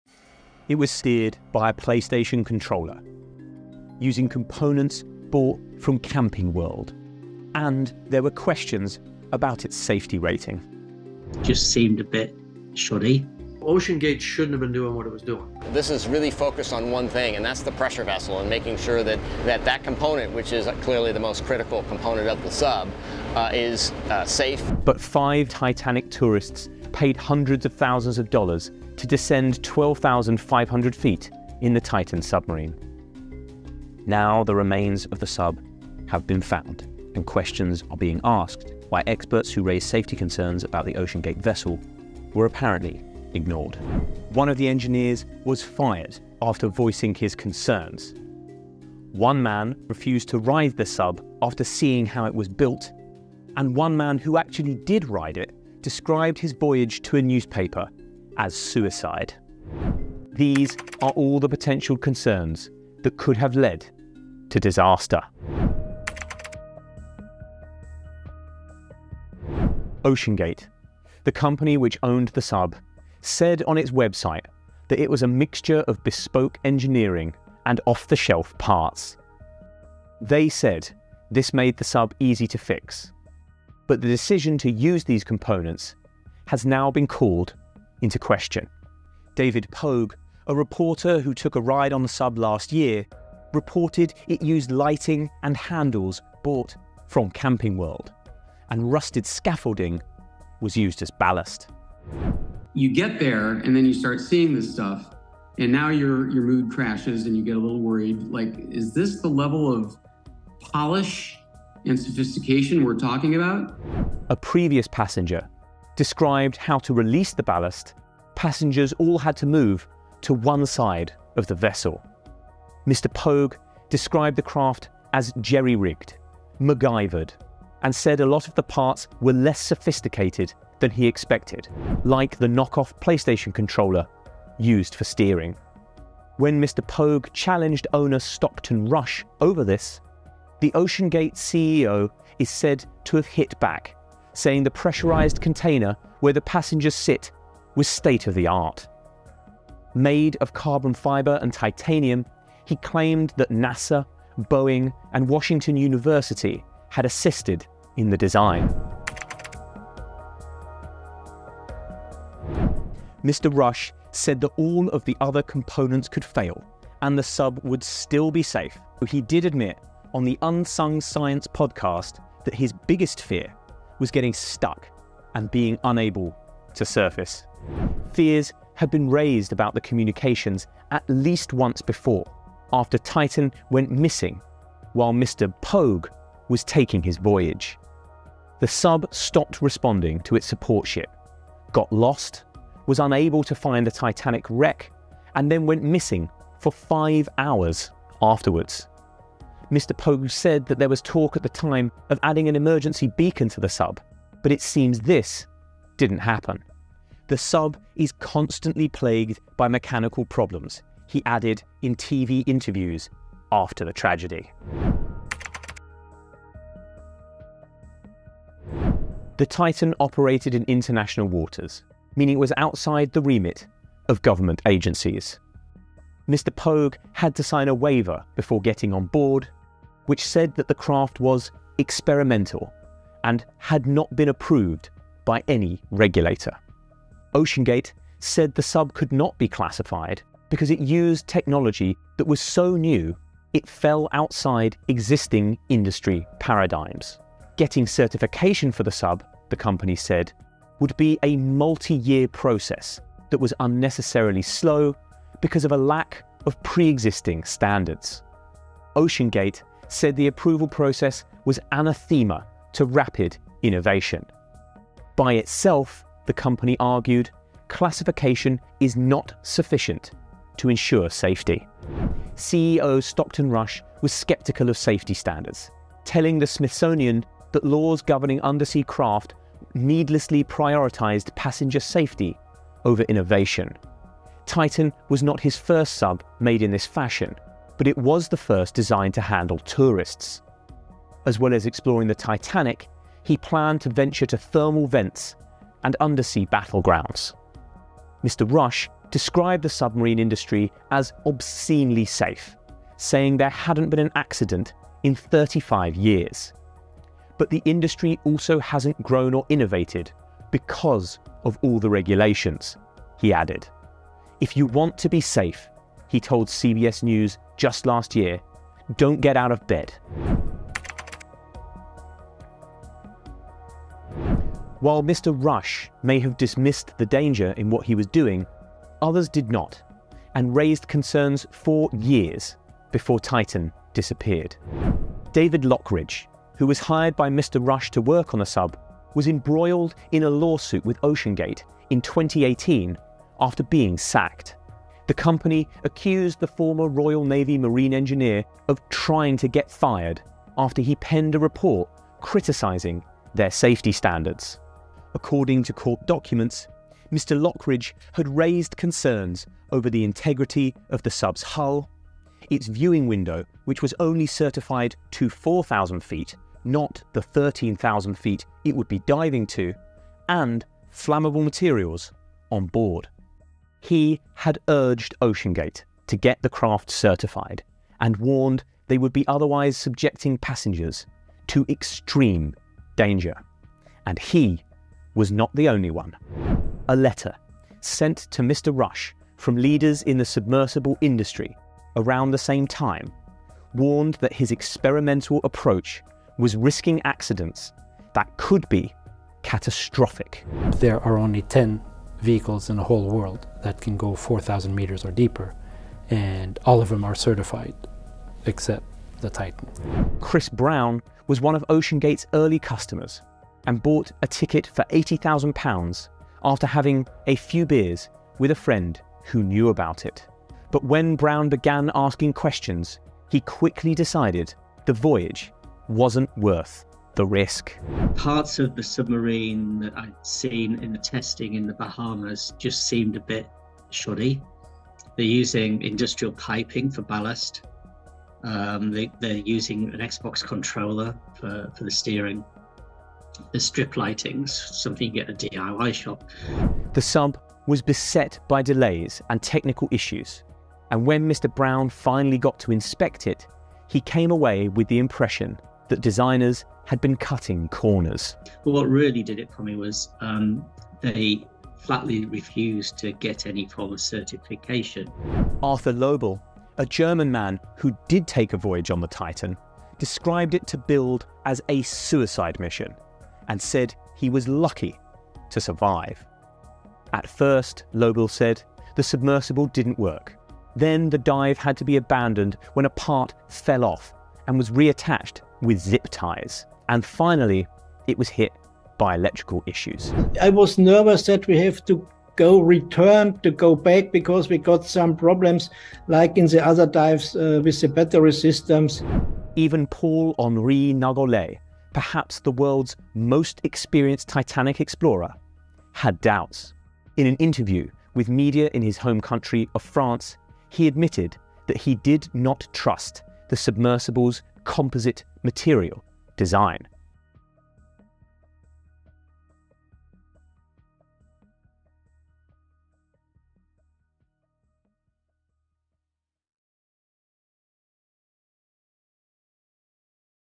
These Titanic submarine 'flaws' may be behind disaster
On 22 June the wreckage of the Titan submarine was found on the sea bed 500 yards from the Titanic. Experts say the submarine suffered a catastrophic implosion and the five passengers onboard died. It was steered by Playstation controller, used components bought from Camping World and didn’t have safety certification. But five missing Titanic tourists paid to descend 12,500ft in the Titan submarine. Now they have vanished without a trace, and questions are being asked why experts who raised safety concerns about the OceanGate vessel were apparently ignored. One of the engineers was so concerned he quit the project, one man was said to have refused to ride the sub after seeing how it was built, and one man who did described his voyage to a newspaper as ‘suicide’. These are all the potential concerns that could have led to disaster.